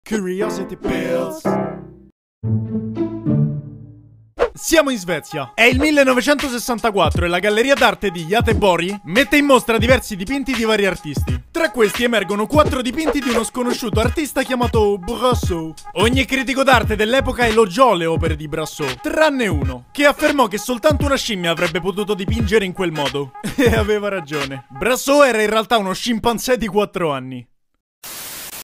Questo artista NON È UMANO!😱
Buon Lunedì Amici!

Ecco a voi Brassau! Uno stranissimo artista che non è UMANO.
Chi dipingerà questi bizzarri dipinti? Scoprilo in questo video!